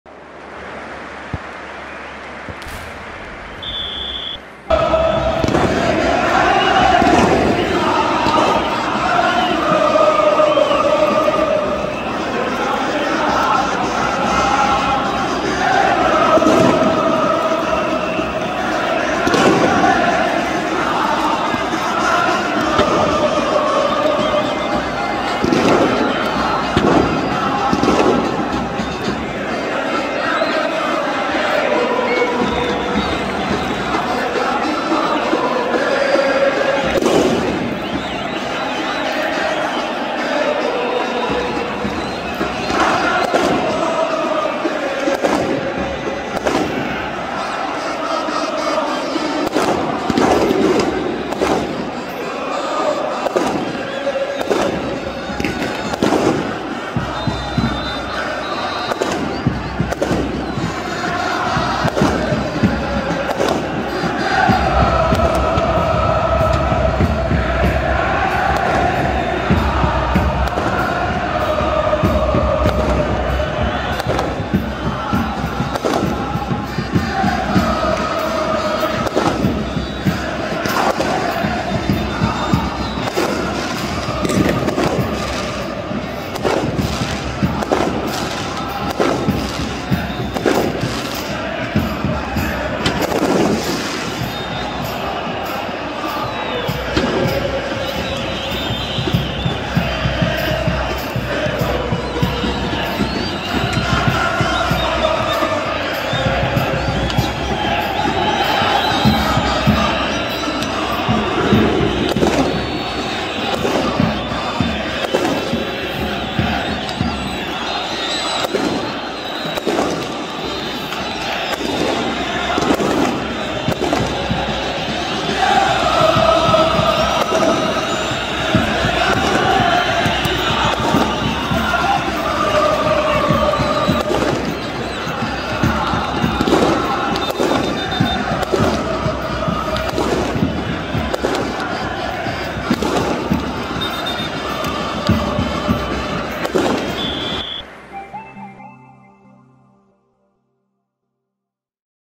جمهور الوداد يشعل نهائي كأس العرش بـ"كراكاج" خيالي!
البطولة | حصريــــــــــــاً